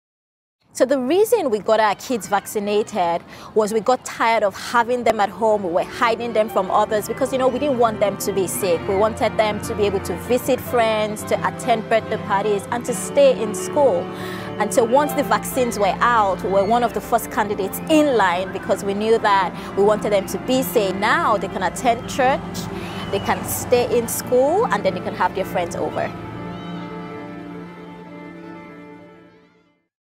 Why I vaccinated my children against COVID-19: Tolu Olumuyiwa
Tolu Olumuyiwa is a mom in Houston who knows it's important for kids to be kids. That's why she chose to get her children vaccinated against COVID-19 and tested. She now has peace of mind when her children hang out with others, attend events, and go to school.